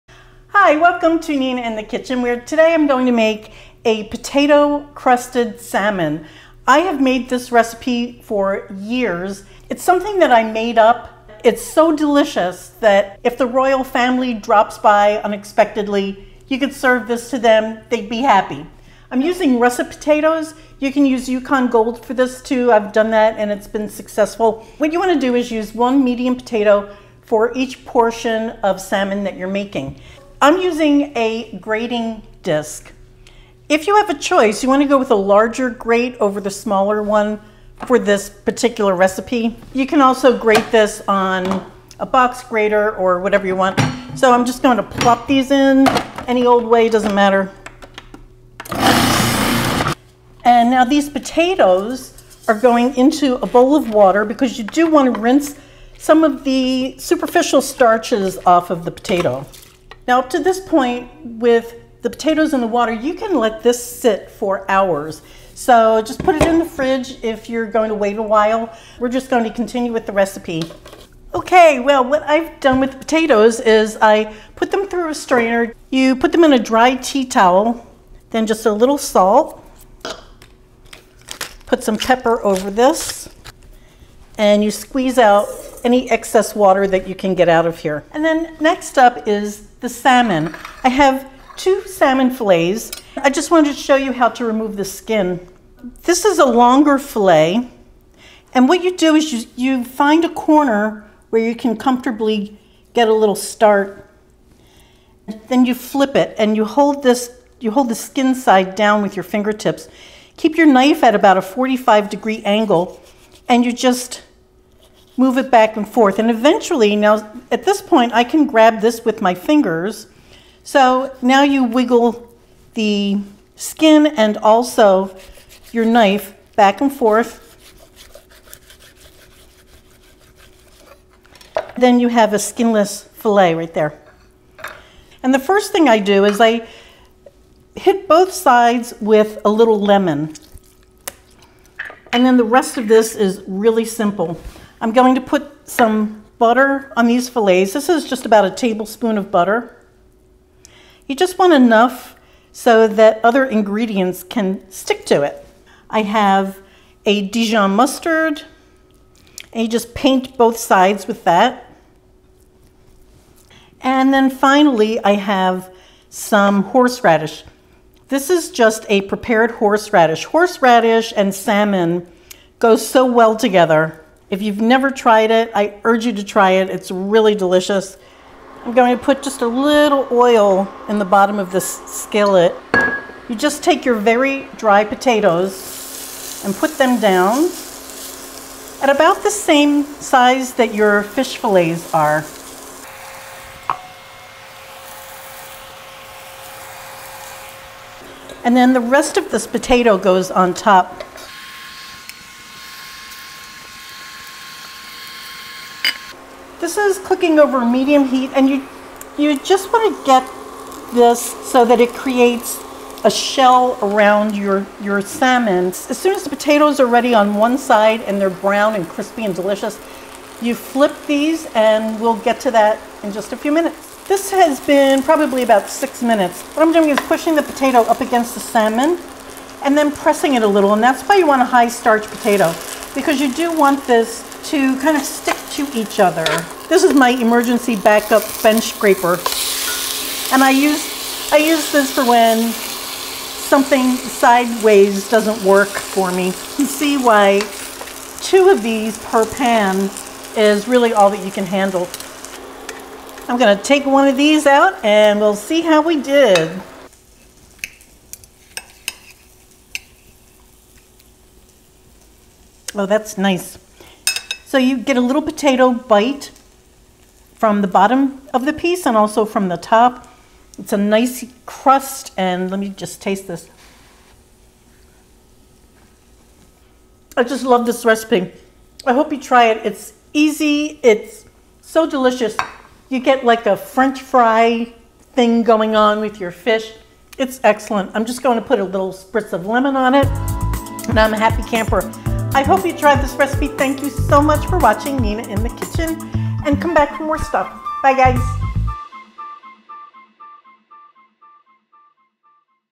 Potato Crusted Salmon
Encased in a crisp potato crust, this salmon is moist and perfectly cooked every time! 😋